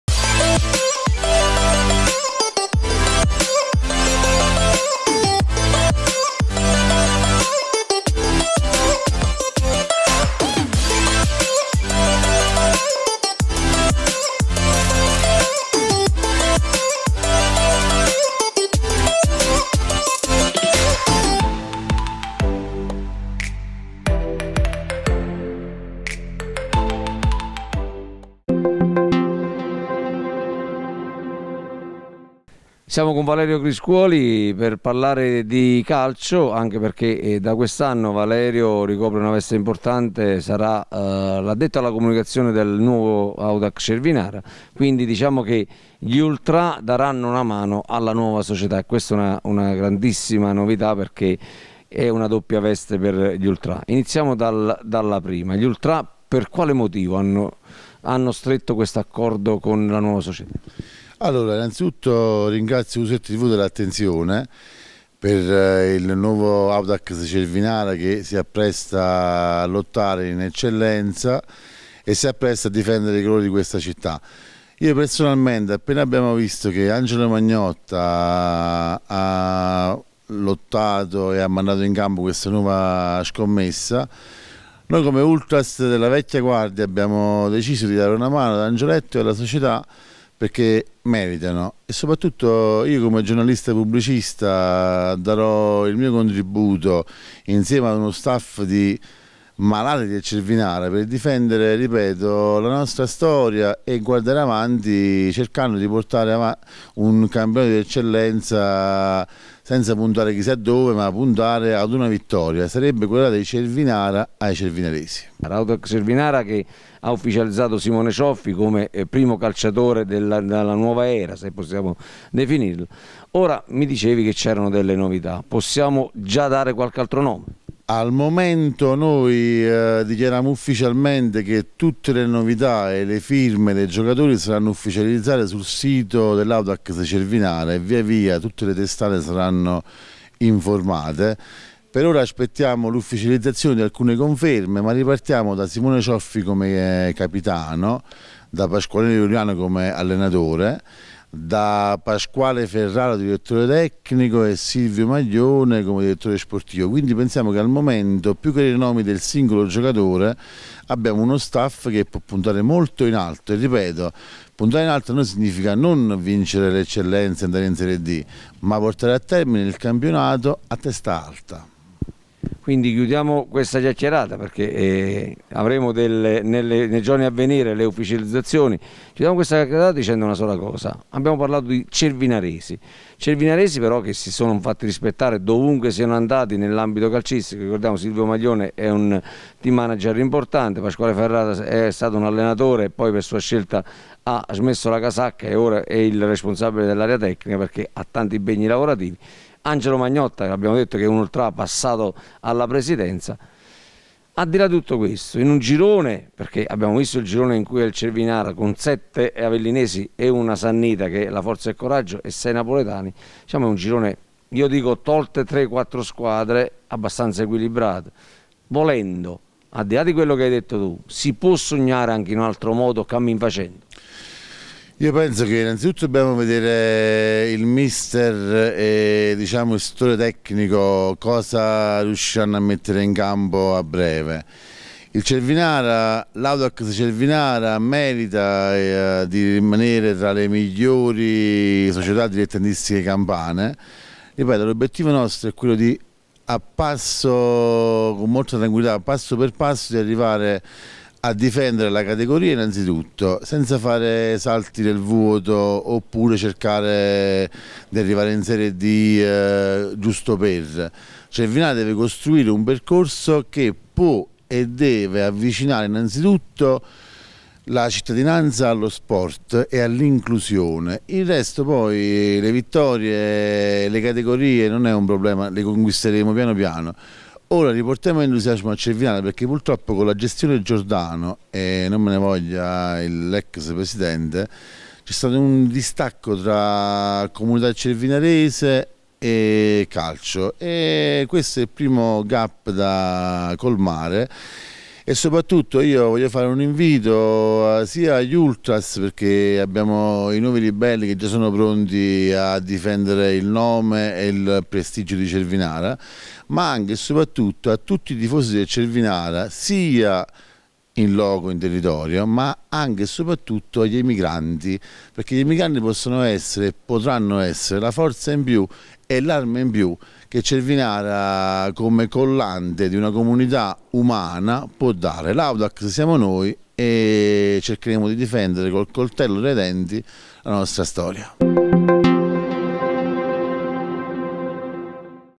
Audax Cervinara. Un progetto che appassiona di nuovo i tifosi
Audax Cervinara. C'è entusiasmo ritrovato con la nuova cordata e anche i tifosi sono parte attiva. Ne parliamo con il nuovo responsabile della comunicazione e storico tifoso  Valerio Criscuoli.